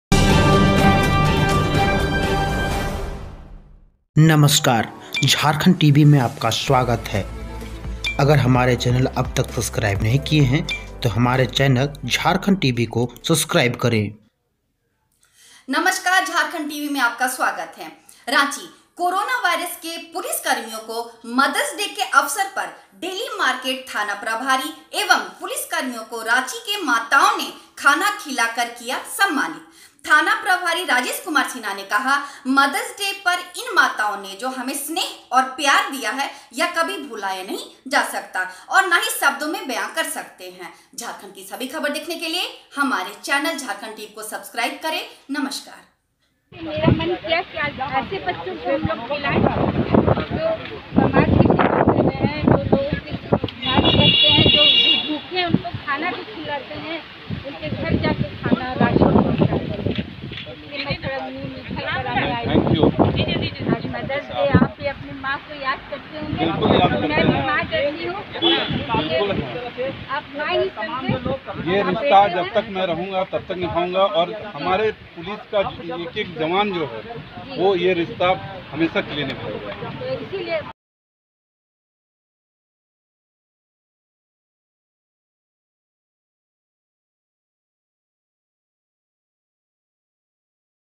पुलिसकर्मियों को रांची के माताओं ने खाना खिलाकर किया सम्मानित
Jharkhand tv, ranchi, bokaro, public, police